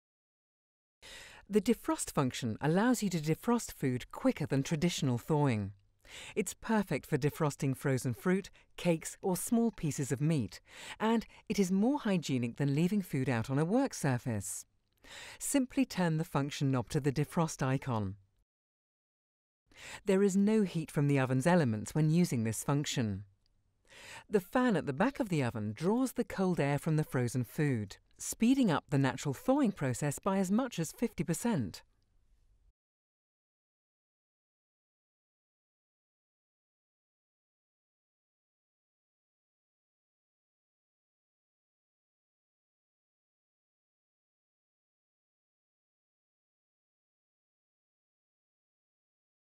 Falcon upright cooker multi-function feature 'Defrost' (fan only)
Selected Falcon upright cookers feature multi-function ovens.  This means you can choose a function to suit your food type.  Learn more about how the feature 'Defrost' (fan only - no element) works in a Falcon freestanding cooker and how you can defrost frozen foods more quickly and safely than conventional methods.